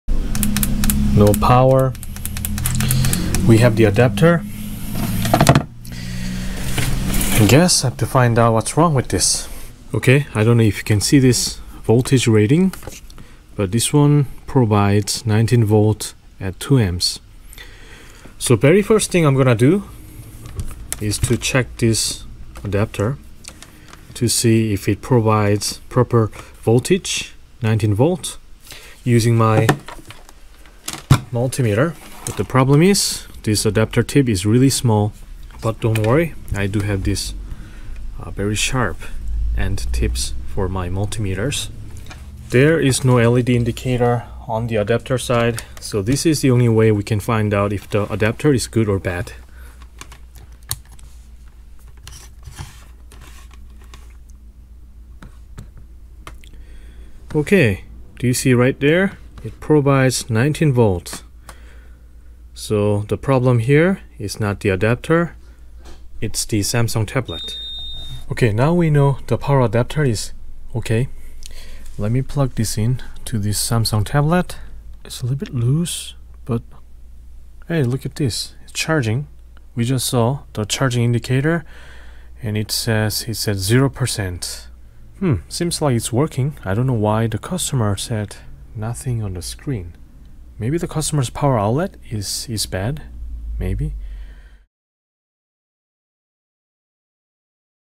How to test power adapter (good or bad) - Laptop, tablet power supply test
Hello Youtube, in today's video, we are going to find out how to check or test power adapter for your laptops and tablet computers. Your computer does not power up all of a sudden? Very first thing you need to check is the power supply unit. It is very common to have a faulty power adapter, but how to test them? All you need is a multimeter, and that's it! Let's find out how to check power adapter with a multimeter in 3 minutes.